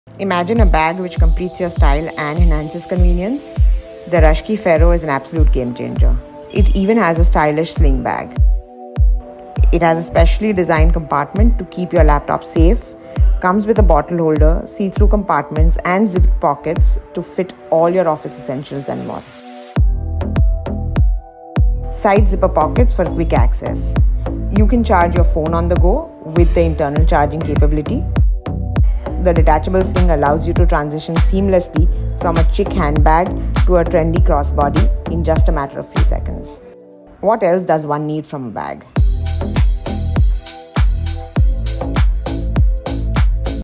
Fero is the perfect travel partner with its in-built SMART features.
Fero is the perfect travel partner with its in-built SMART features. This vegan charmer strikes a balance between functionality and style so that you can move around hassle free.